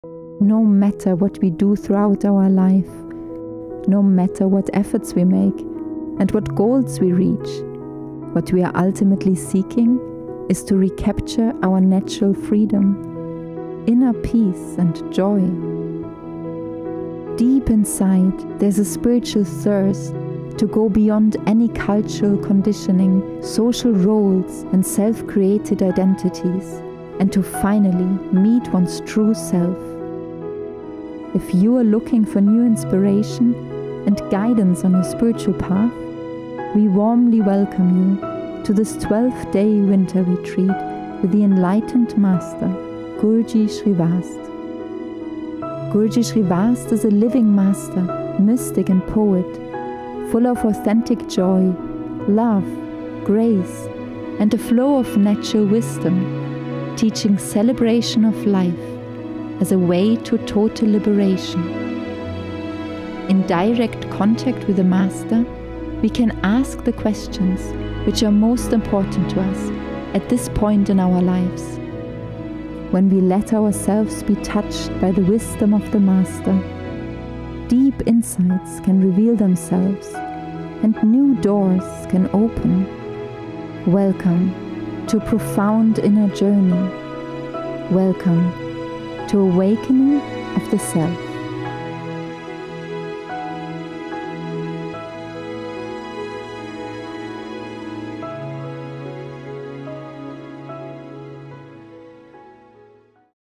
Upcoming Winter Retreat 'Awakening of the Self' with Guruji Sri Vast, in Sweden, 22 Dec - 2 Jan
Welcome to the upcoming Winter Retreat with Guruji Sri Vast.
Throughout life, no matter what we do, all efforts are made with the ultimate goal of finding one's total freedom - the liberation - to recapture one's natural inner peace, love and joy. This is the spiritual thirst to go beyond old social and cultural conditionings and to explore the very core of one's existence.

A retreat with Guruji Sri Vast is a very profound, transformative experience. It is an opportunity to immerse oneself in the graceful presence of the Master, on the path of total liberation – the Enlightenment.

It is an invitation for Awakening - to meet one's true Self.

'Awakening of the Self'
22 Dec - 02 Jan
in Divinya, south of Sweden